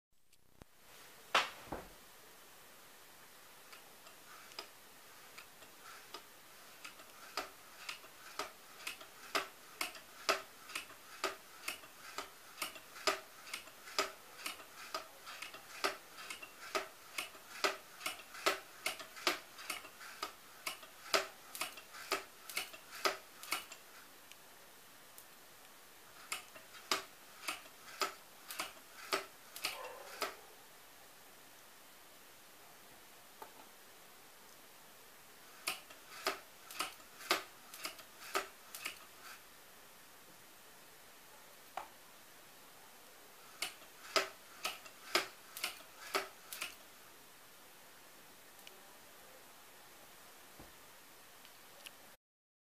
Double electrophorus machine
This electrostatic machine based on two variable capacitors is equivalent to Wilson's machine, but has a symmetrical structure.